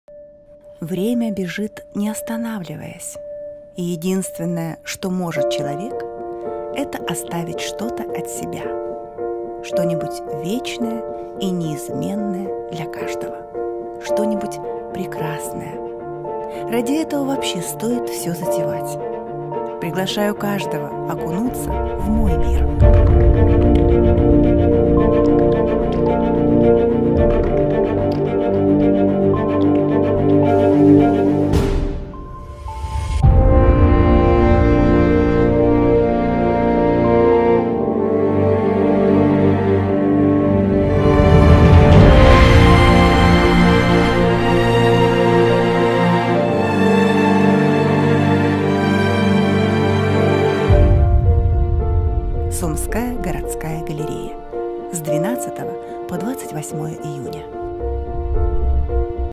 Event promo Gallery (VideoEditing.today)
Promo video for Event of talented ukrainian artist Anastasiya Martynenko
Editor: Alexander Parfilo
If you need a team of professional video editors who will gather footage in high-quality video, go to: